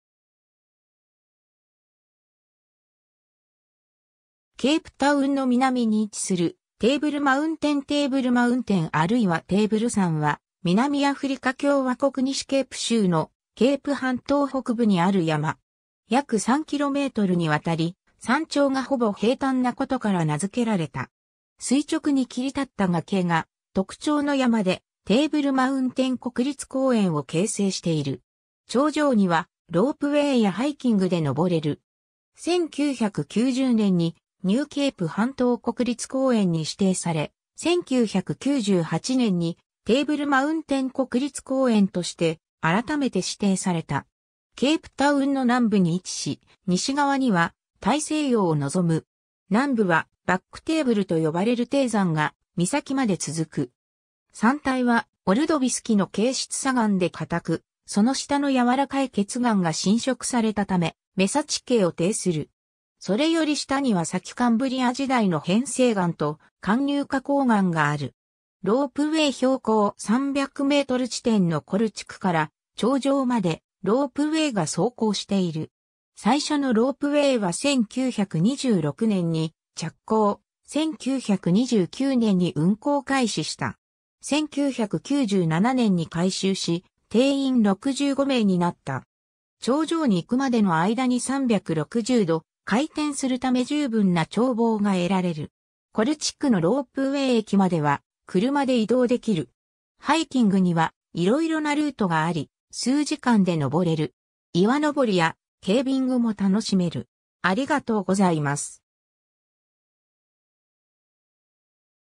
テーブルマウンテン (南アフリカ)
ケープタウンの南に位置するテーブルマウンテン（右が南） テーブルマウンテン（英語: Table Mountain、アフリカーンス語: Tafelberg）あるいはテーブル山は、南アフリカ共和国西ケープ州のケープ半島北部にある山。
約3kmに亘り山頂がほぼ平坦なことから名づけられた。
 垂直に切り立った崖が特徴の山で、テーブルマウンテン国立公園を形成している。
頂上にはロープウェイやハイキングで上れる。
 1990年にニューケープ半島国立公園に指定され、1998年にテーブルマウンテン国立公園として改めて指定された。
 ケープタウンの南部に位置し、西側には大西洋を望む。
南部はバック・テーブルと呼ばれる低山が岬まで続く。
山体はオルドビス紀の珪質砂岩で硬く、その下の柔らかい頁岩が浸食されたためメサ地形を呈する。
それより下には先カンブリア時代の変成岩と貫入花崗岩がある。
 ロープウェイ 標高300m地点のコル地区から頂上までロープウェイが走行している。
最初のロープウェイは1926年に着工、1929年に運行開始した。
1997年に改修し定員65名になった。
頂上に行くまでの間に360度回転するため十分な眺望が得られる。
 コル地区のロープウェイ駅までは車で移動できる。
 ハイキングにはいろいろなルートがあり、数時間で上れる。
岩登りやケービングも楽しめる。